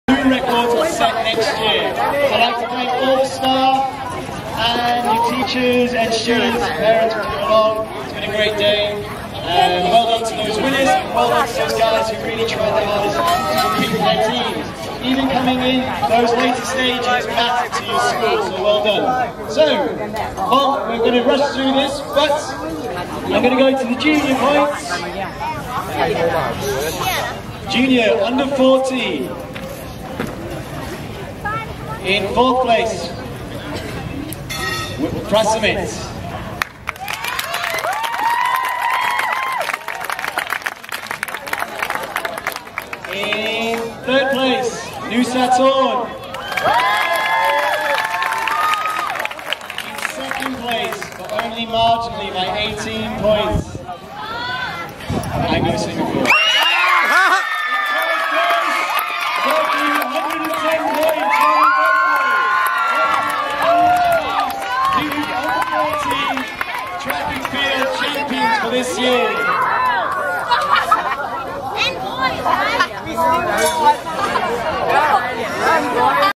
Berkeley's JV Win the Trophy!
It's officially announced as Berkeley win the 14 and under GBAC track & field meet. Well done to all our athletes, staff and parents for making this such a special afternoon of pure competition and team spirit! Sports. We are Berkeley!